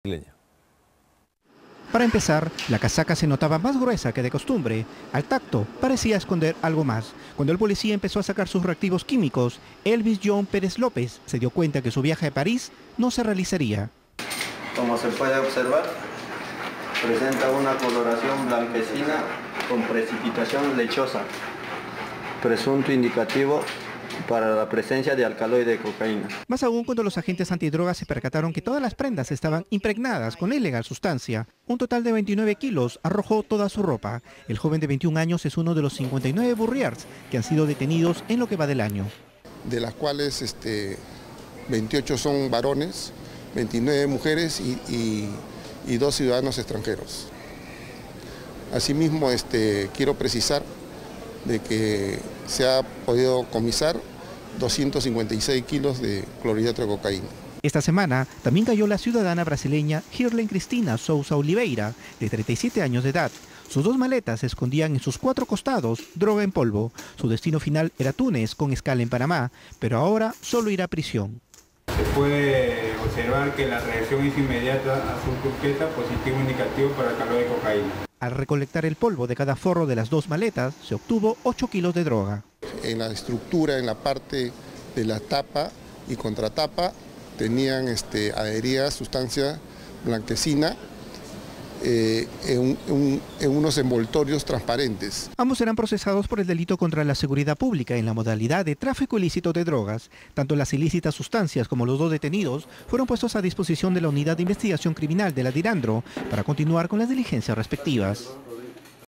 Aeropuerto Jorge Chávez: Burriers llevaban droga camuflada en ropa y maletas
En un operativo reciente en el Aeropuerto Jorge Chávez, las autoridades detuvieron a Elvis Pérez López, de 21 años, cuando intentaba viajar hacia París.
Los agentes antidrogas se percataron de que su casaca y otras prendas de vestir estaban impregnadas con una sustancia ilegal. Tras realizar pruebas químicas, se confirmó que los 29 kilos de polvo blanco presentes en la ropa contenían alcaloide de cocaína.